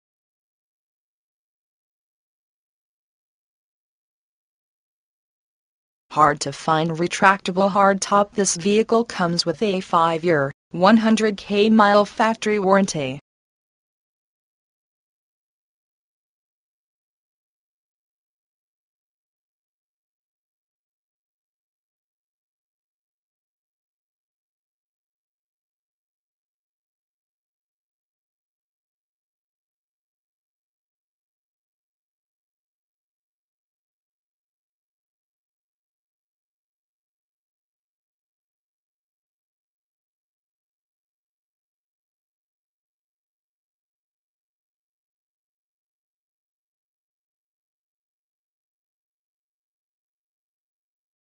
2010 Chrysler Sebring - Barnett Chrysler Jeep Kia Used
For Sale: 2010 Chrysler Sebring
VIN: 1C3BC6EV6AN170511
Engine: Gas V6 3.5L/215
Drivetrain: FWD
Transmission: Automatic
Mileage: 15,254
Color: Inferno Red Crystal Pearl (exterior) Dark slate gray (interior)

Features:
HARD TO FIND RETRACTABLE HARD TOP!  THIS VEHICLE COMES WITH A 5-YEAR, 100K-MILE FACTORY WARRANTY!